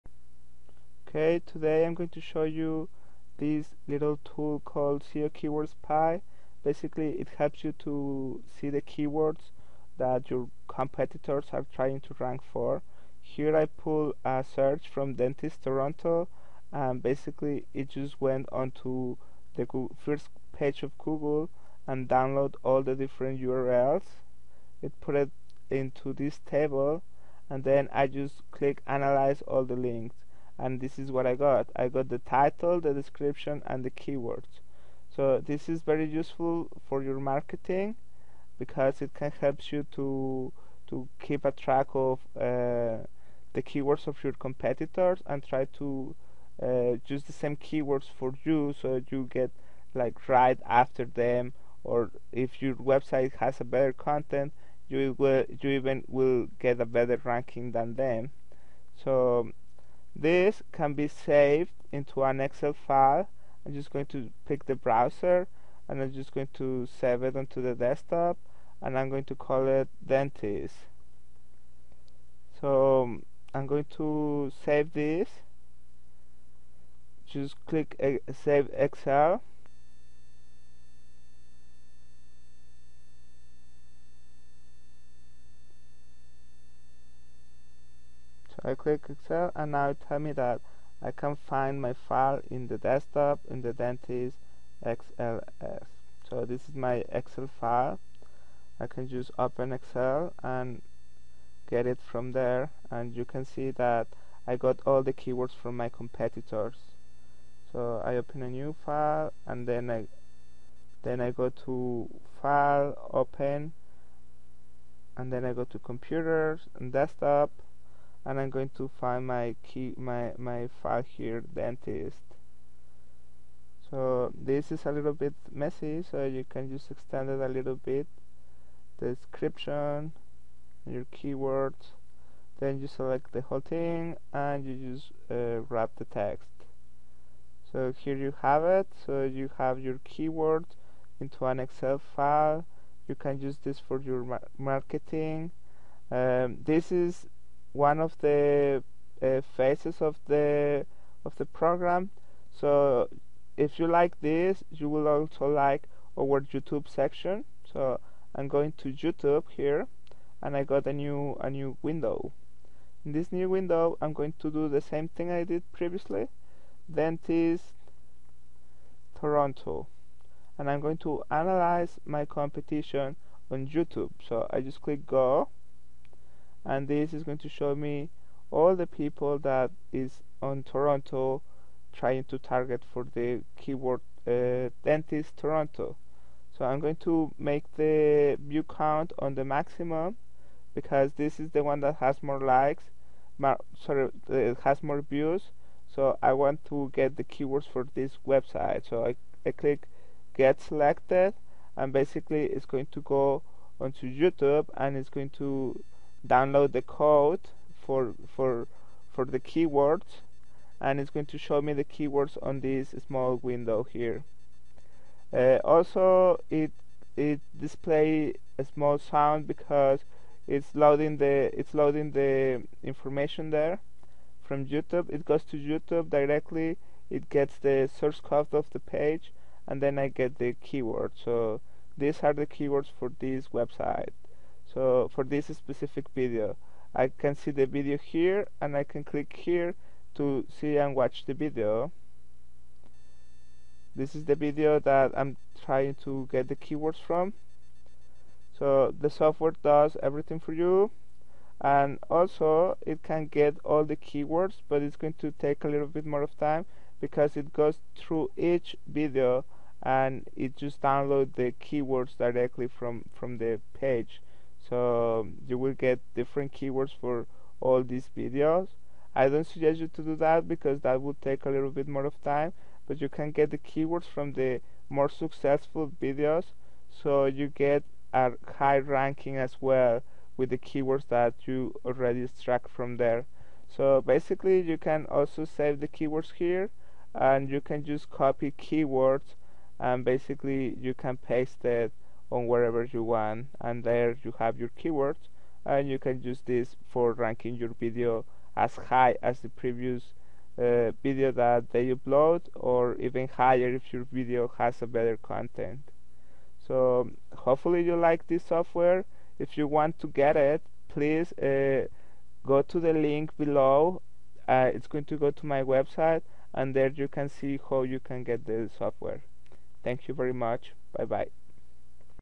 Spy Keyword - Keyword competitor and competition analysis
SEO Keyword Spy -Analize the keywords of your competitors in google and Youtube